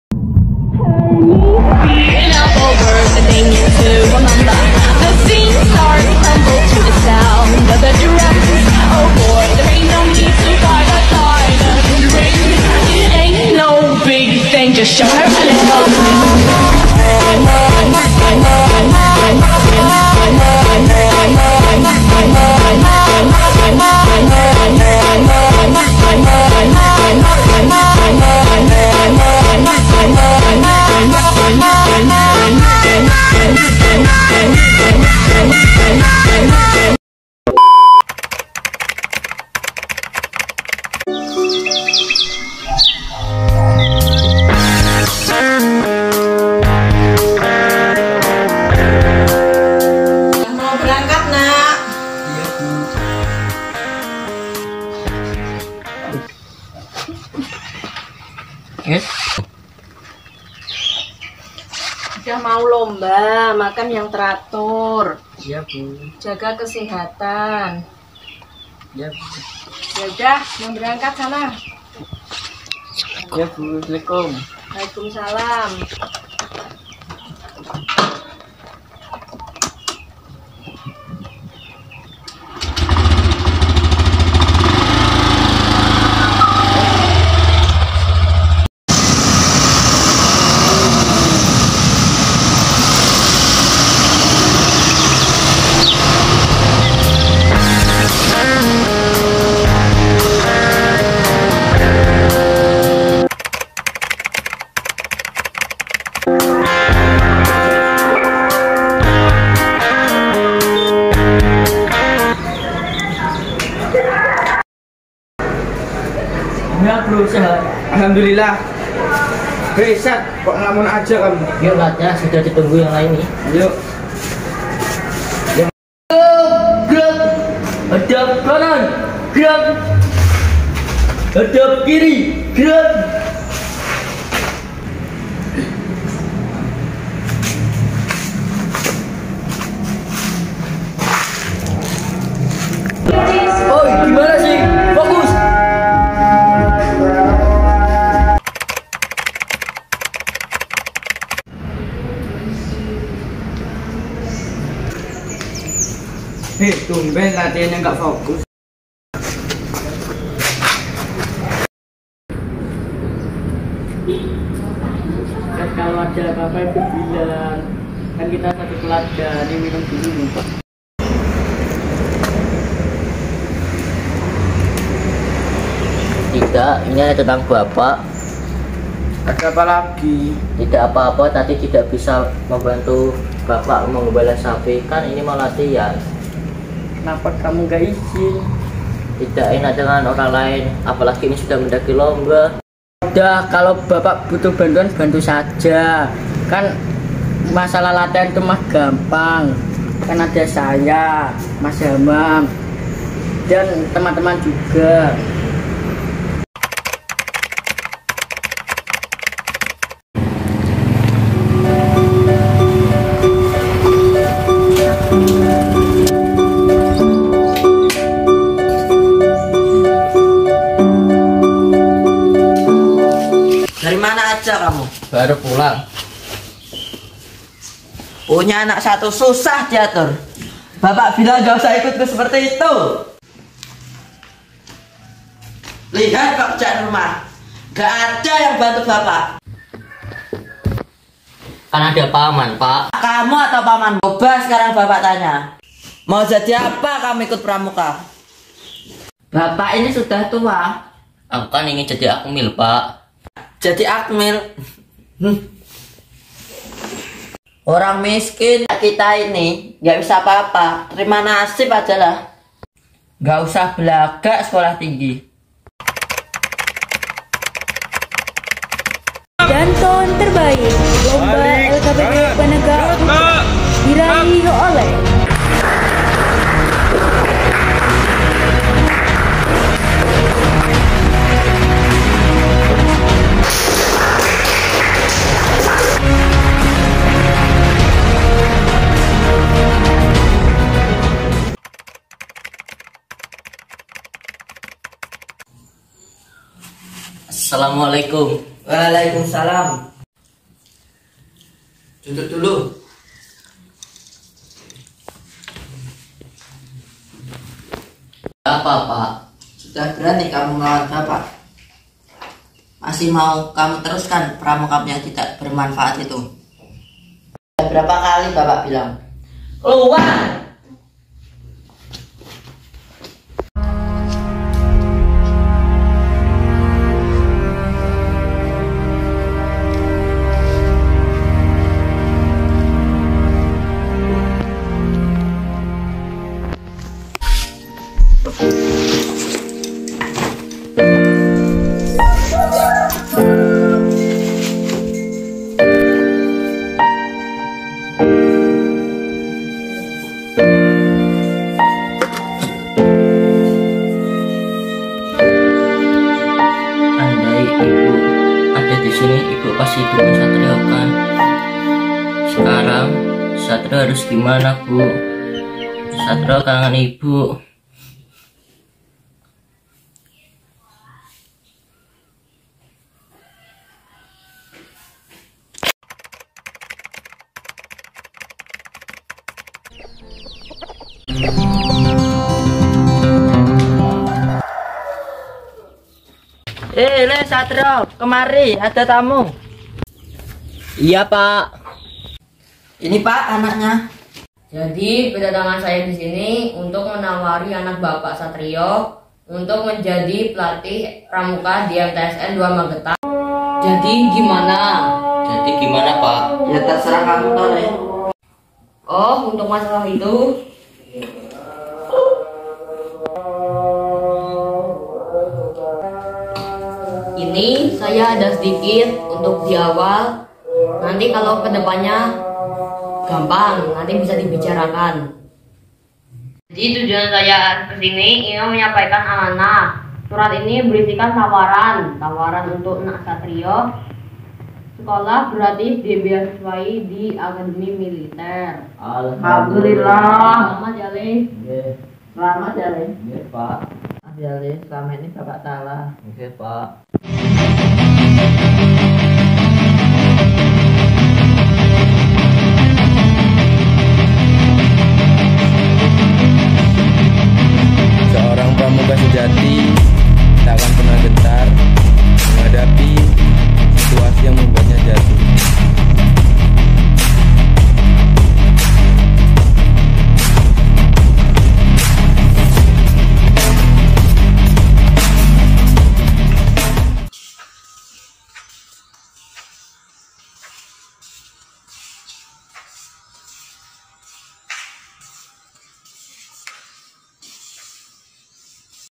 Film Pendek Siswa | Drama (Satrio dkk)
Film pendek ini diproduksi oleh siswa-siswi MTsN 2 Magetan untuk memenuhi tugas mata pelajaran Bahasa Indonesia.